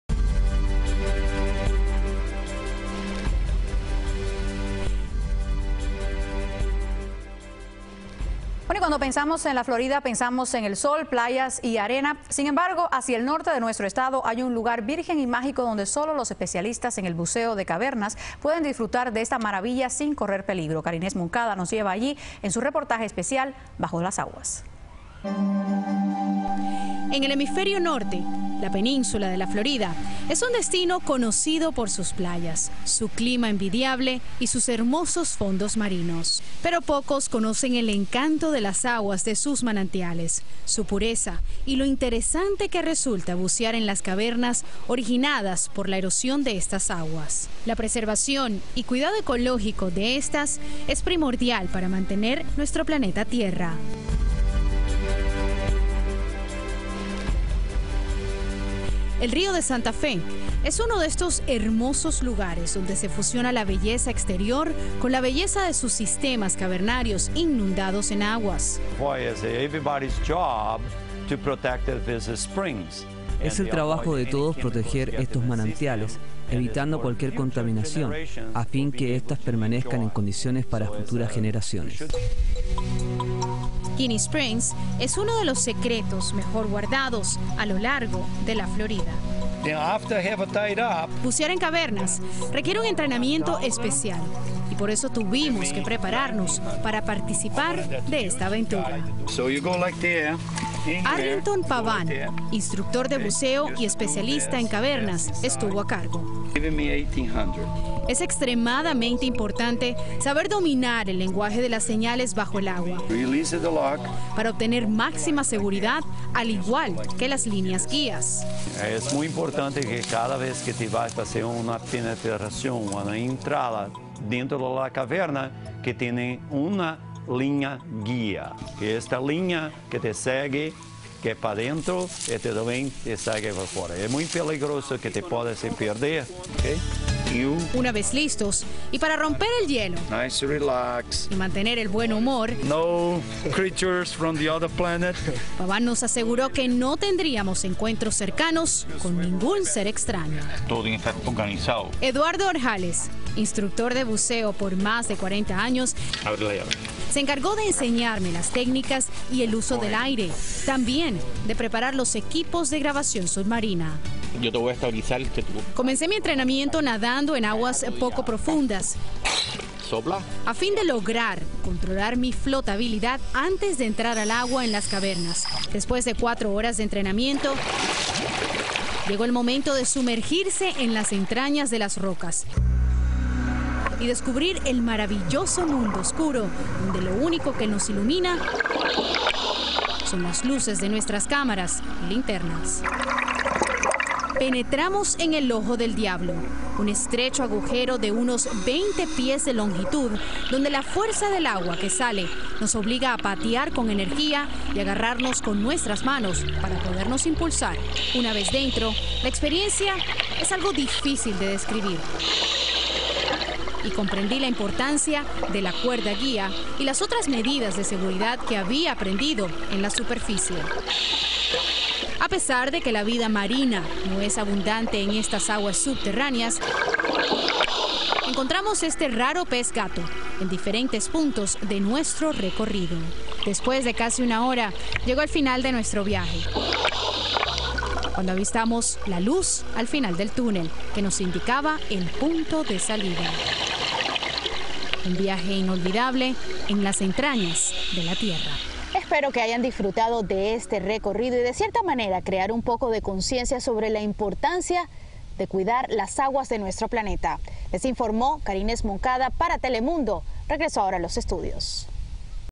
Ginnie Springs on Telemundo with Dixie Divers
Pavan Cavern instructor with Telemundo crew cover the aspects of cavern Diving.